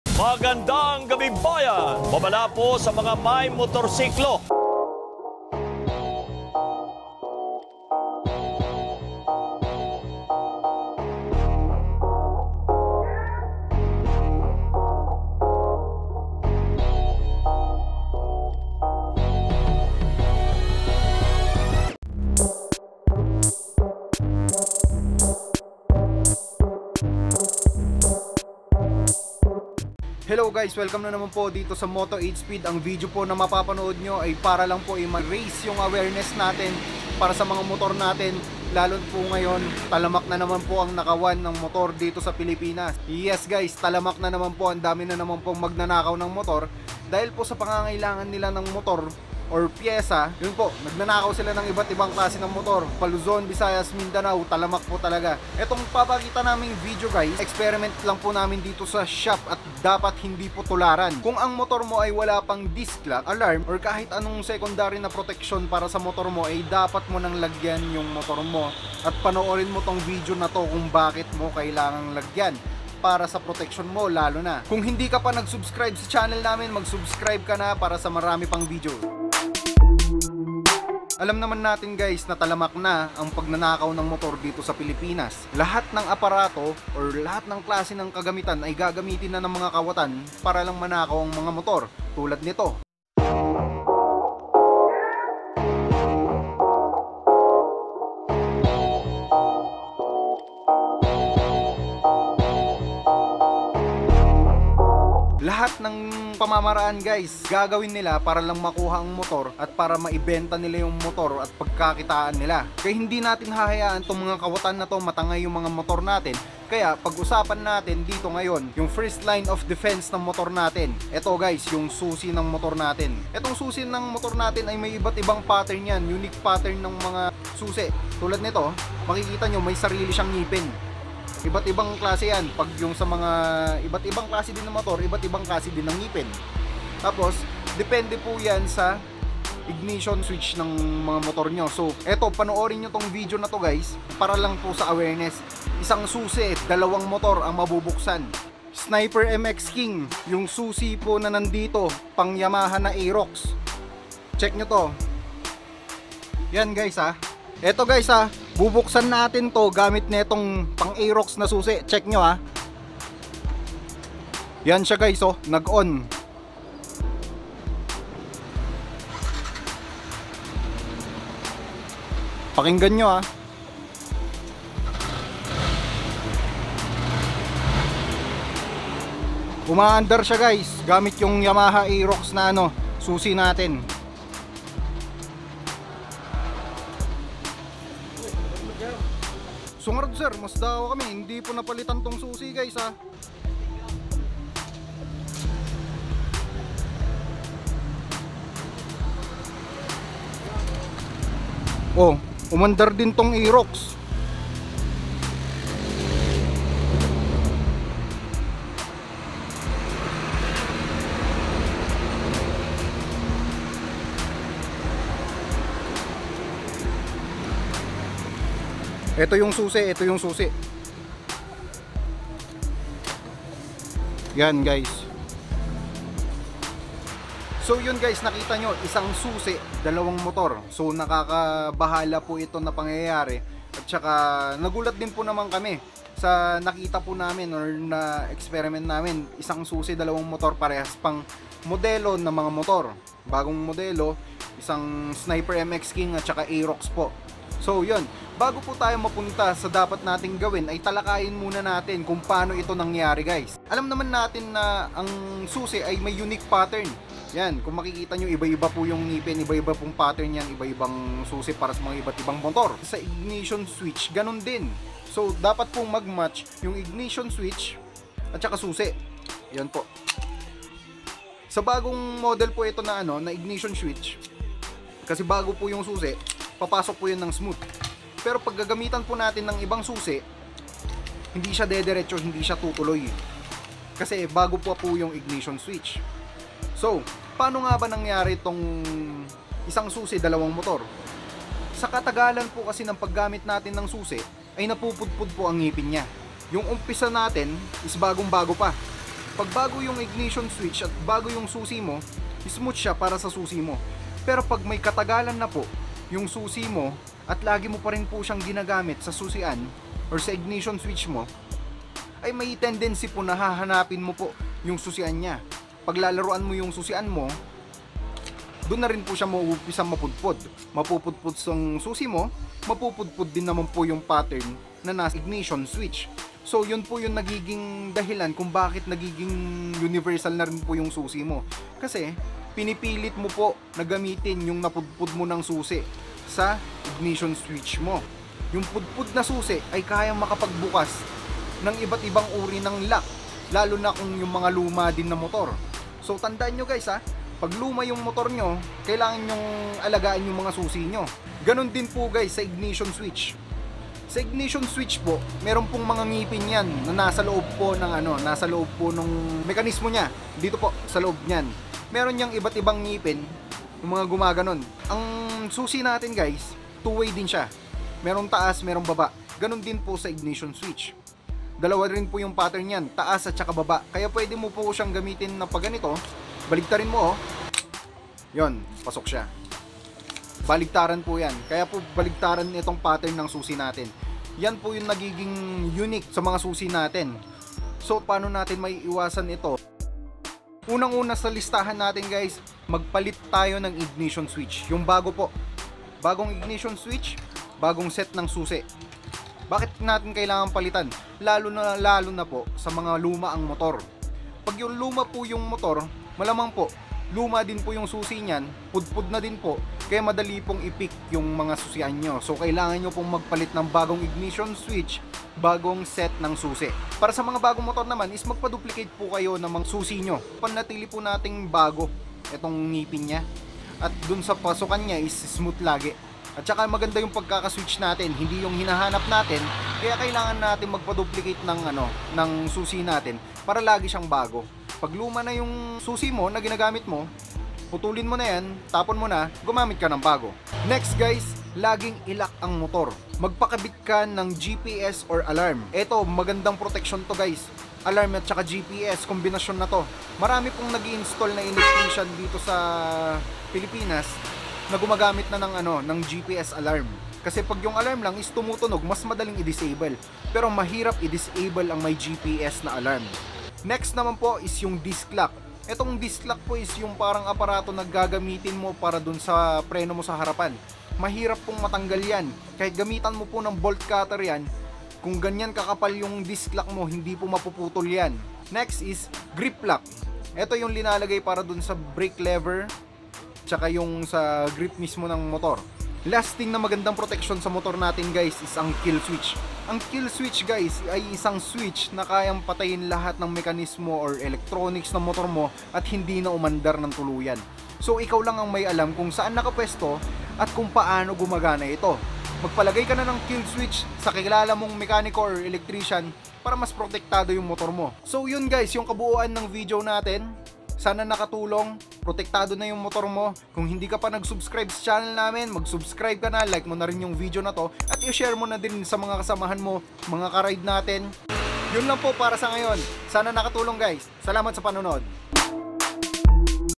Isang Susi Dalawang Motor | Paalala para Iwas Nakaw Motor (Yamaha Motorcycles)| Moto 8Speed
Siguruhing ligtas ang inyong motor.
Isang Susi dalawang motor.
Magpalagay ng
GPS Alarm
Anti Theft Alarm
Disc Lock
Grip Lock
Kill Switch
at iba pang karagdagang proteksyon para maiwasan ang pagkanakaw ng motor.